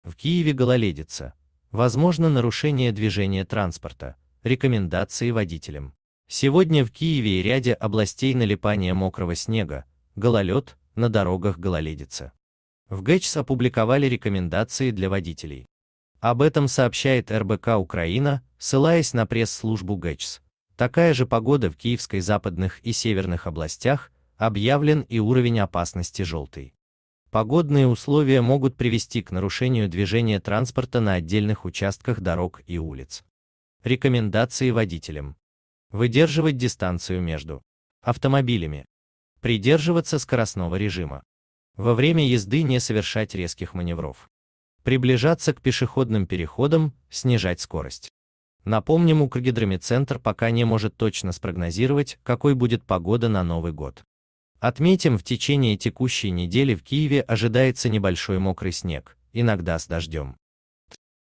В Киеве гололедица. Возможно нарушение движения транспорта: рекомендации водителям.
Киев ПогодавКиеве Сегодня в Киеве и ряде областей налипание мокрого снега, гололед, на дорогах гололедица. В ГСЧС опубликовали рекомендации для водителей. Об этом сообщает РБК-Украина, ссылаясь на пресс-службу ГСЧС. Такая же погода в Киевской, западных и северных областях, объявлен І уровень опасности, желтый). Погодные условия могут привести к нарушению движения транспорта на отдельных участках дорог и улиц. Рекомендации водителям:. выдерживать дистанцию между автомобилями;. придерживаться скоростного режима;. во врем